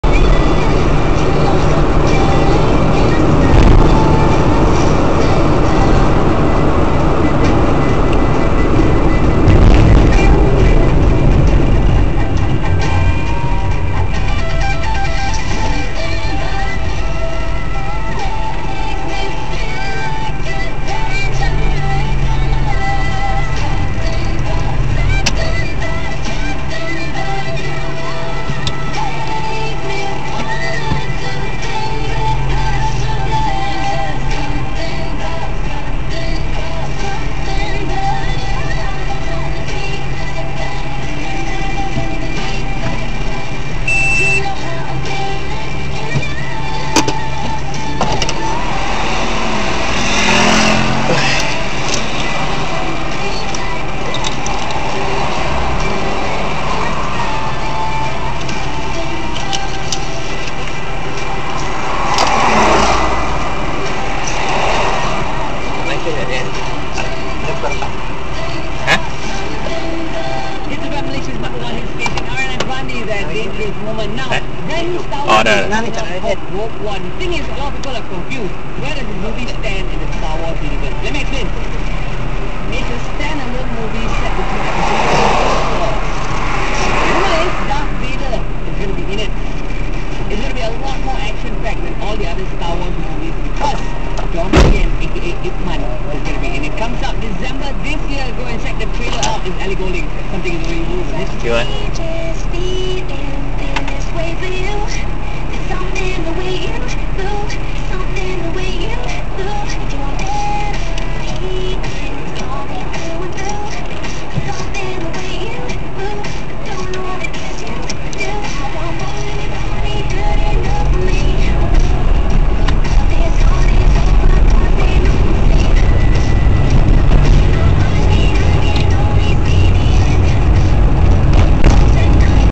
LMST Vlog : OH NO! JPJ TAHAN my LMST during road block o_0 !!
Just another day to work, and met a JPJ road block, they seem like interested to my little LMST! asking me to open the bonner, checking around my car etc, but did not issue me any summon, ( I have illegal number plate font, dark tinted, etc). What a lucky day.